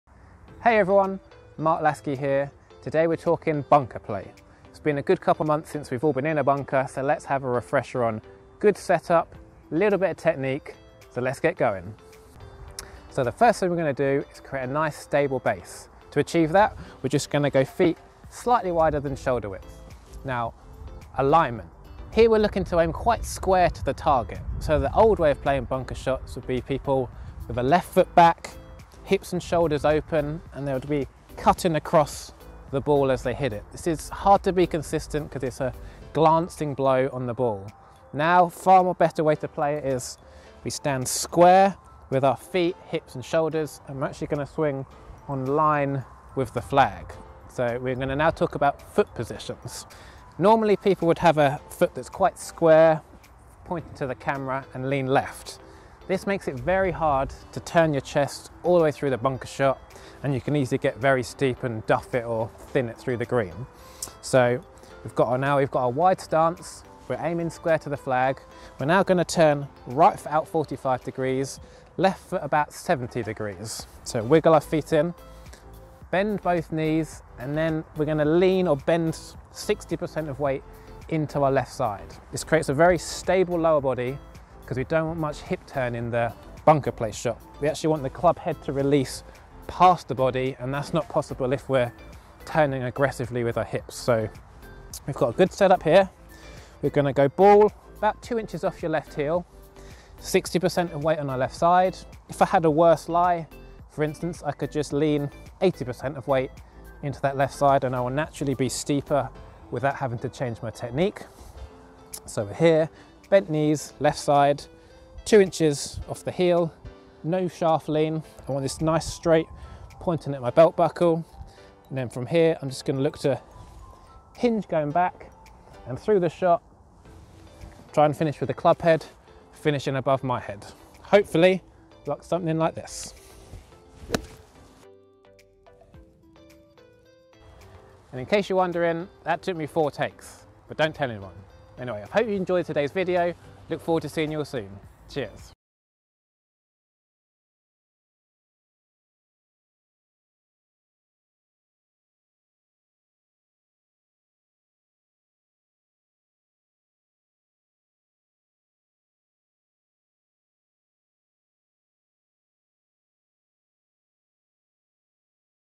Bunker Shots With Mark Laskey
Here we have Mark Laskey going through the different ways to hit a bunker shot and how he recommends what you should be doing!